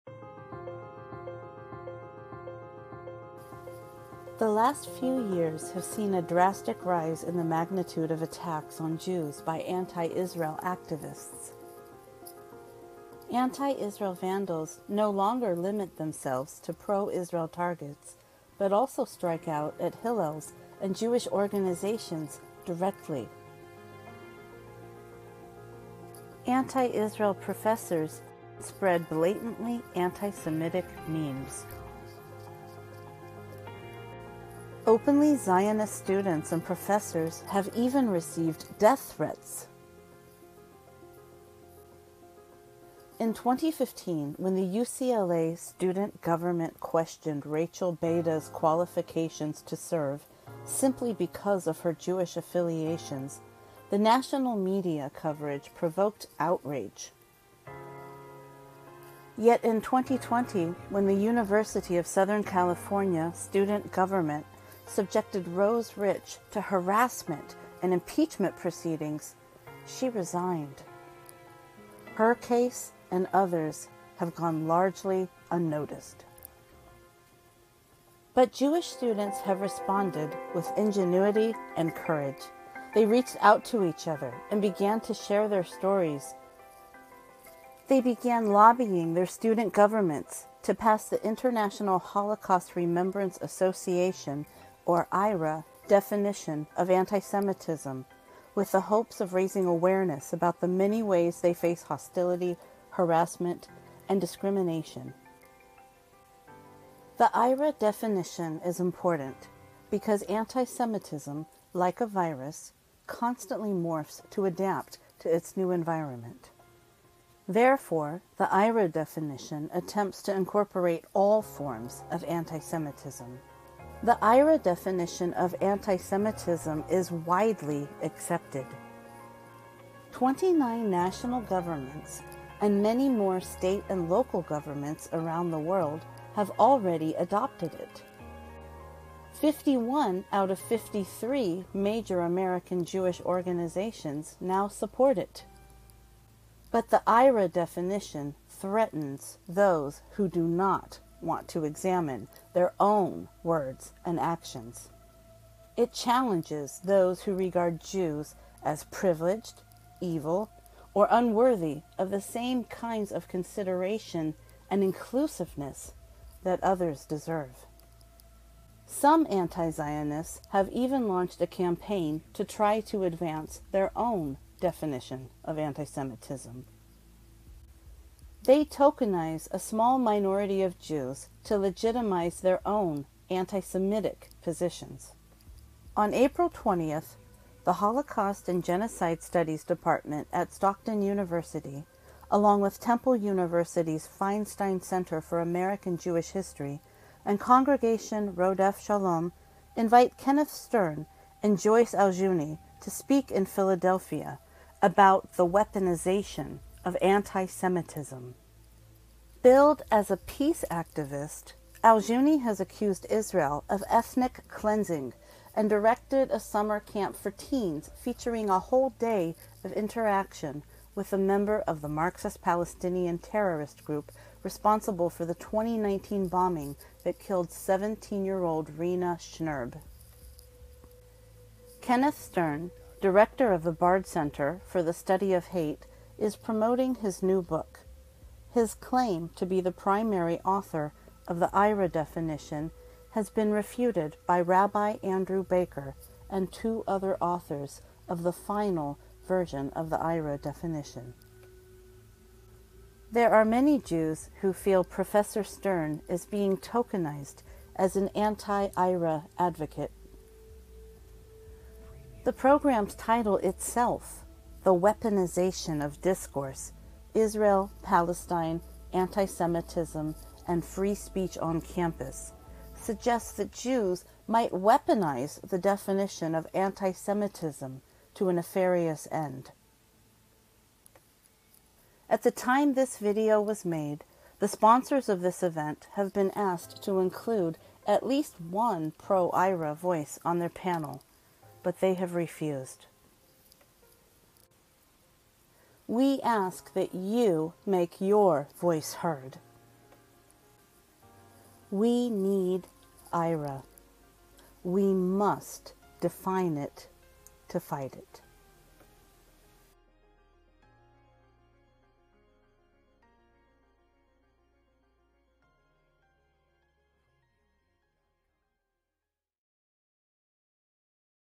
Stockton Holocaust and Temple Jewish Center Attacks on Zionists Not Antisemitism
On April 20, the Holocaust and Genocide Studies at Stockton University, Temple University’s Feinstein Center for American Jewish History, and Congregation Rodeph Shalom in Philadelphia invite Ken Stern and Joyce Ajlouny to speak.

This event promotes anti-Semitism by legitimizing the efforts of non-Jews and anti-Israel activists to define anti-Semitism for Jews.

This video provides a short overview explaining how the event endangers Jewish students on campus.

Holocaust, Genocide, and Jewish students centers  and synagogues should not become a venue for the promotion of anti-Semitism.